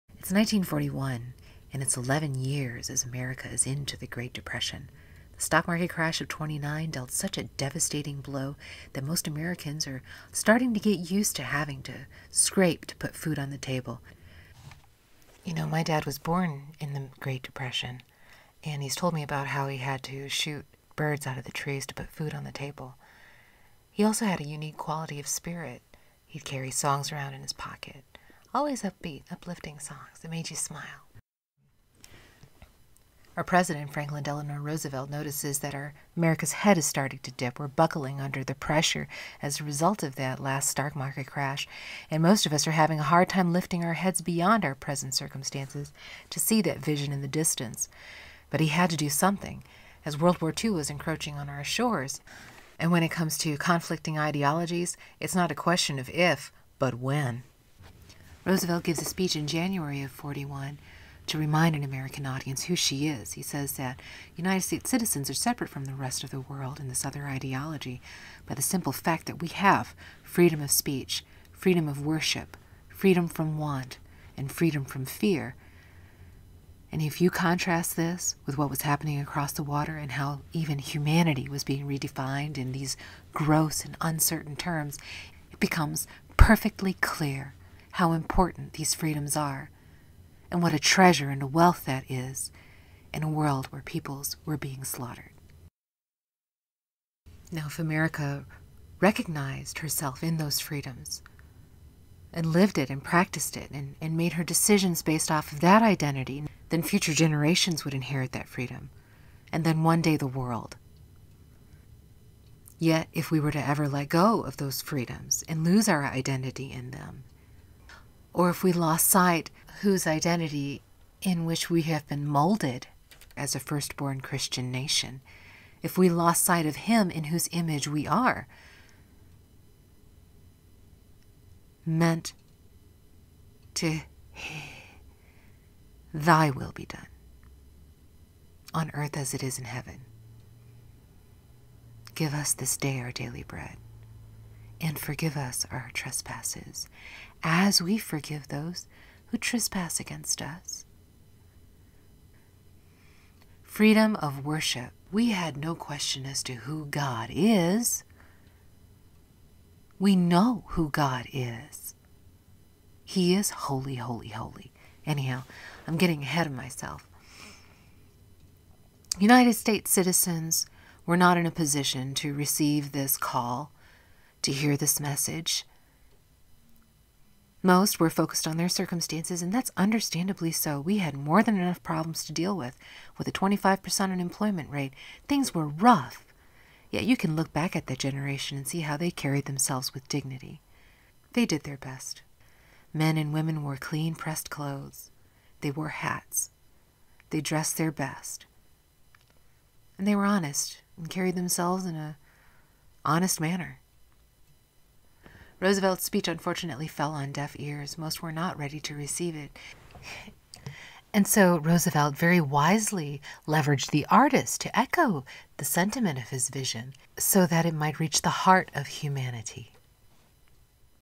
ROSEBUD: A Willing Spirit (1)
Thanks for trying out another of my Audio Sketchbooks.     Here's to One Nation Under God, cause that's what i'll take to exist through time.   I have long-desired to lend voice to my perspective, as I currently have the priveledge to host and learn about the Rockwell Roosevelt and the Four Freedoms exhibit at The Henry Ford.   Here's to that first Amendment right : Freedom of Speech. A rant in relation and support of our current exhibit :